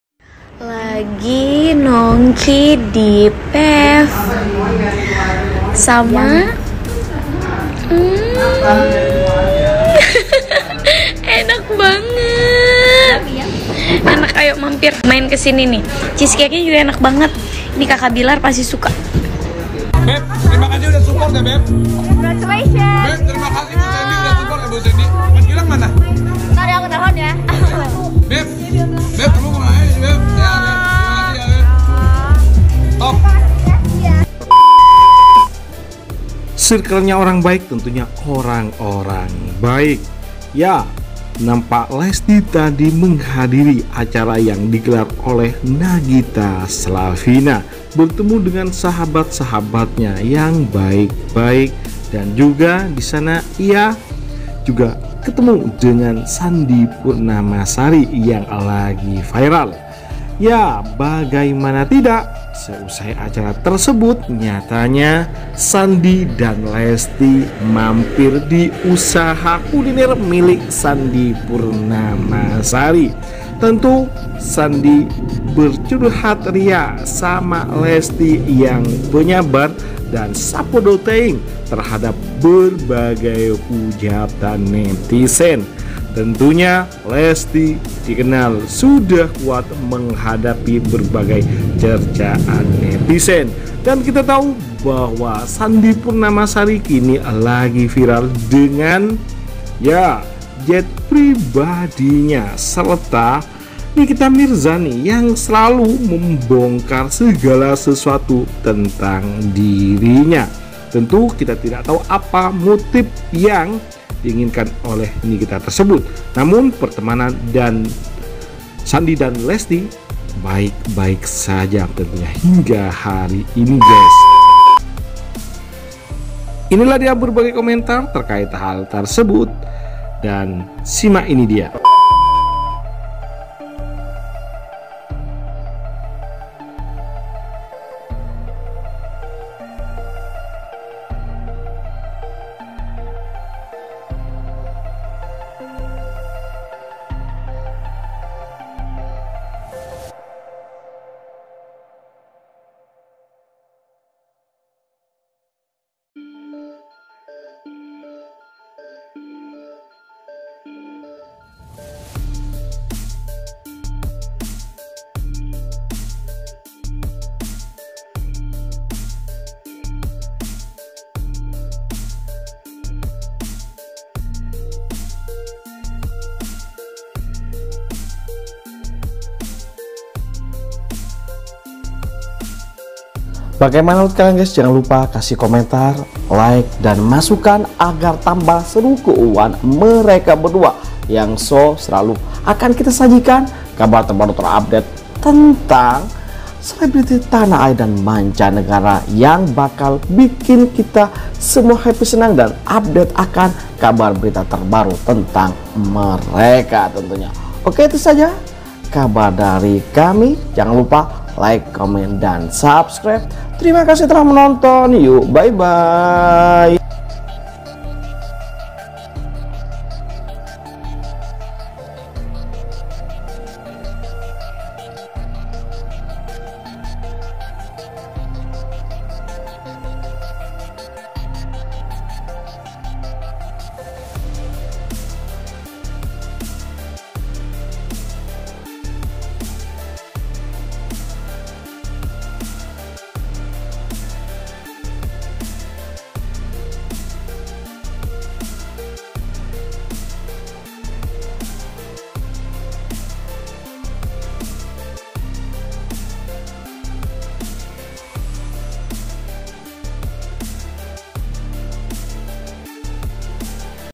Istri Baik❗ Makan Enak Lesti Ingat Suaminya Dirumah❓ Bang Billar Dibungkusin❓
Jangan Lupa: _
Thankz!!..